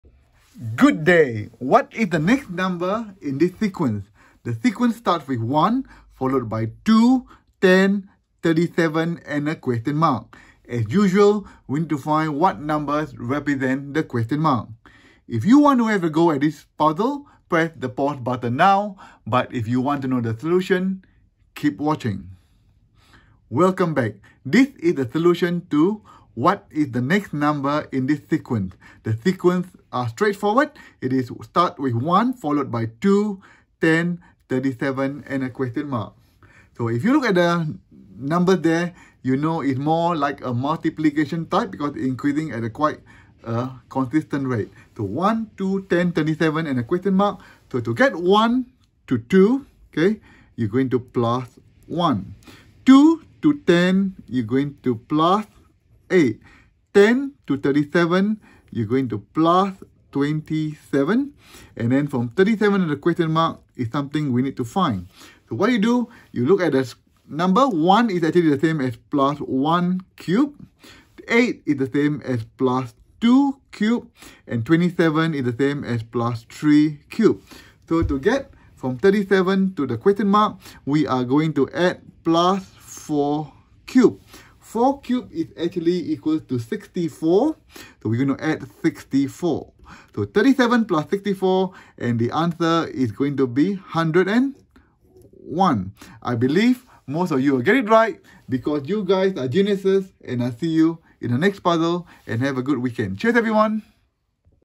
What is the Missing Number in this Sequence?
In this video, we will guide you through the process of solving a challenging math puzzle. Whether you're a puzzle enthusiast or looking to enhance your problem-solving skills, this step-by-step tutorial will help you crack the code and find the solution.

Remember, solving math puzzles requires patience, critical thinking, and a systematic approach. Our tutorial will provide you with valuable insights and strategies to tackle not only this puzzle but also other math puzzles you may encounter in the future.

So, if you're ready to challenge your mind and embark on an exciting puzzle-solving journey, watch this video now and let's crack this math puzzle together!